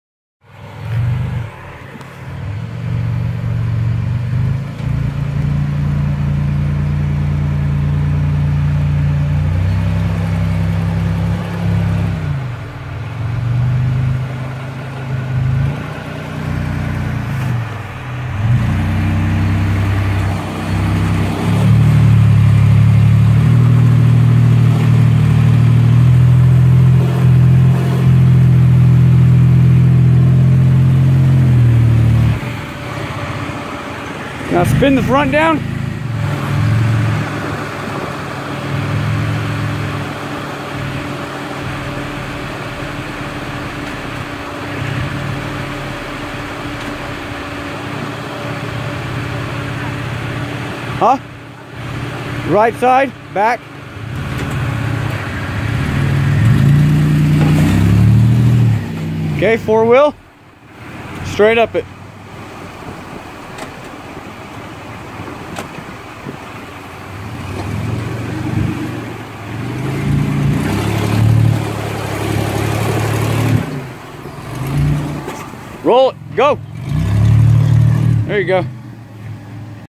Joint effort
Sami driving on joint effort 2017 winter 4x4 jamboree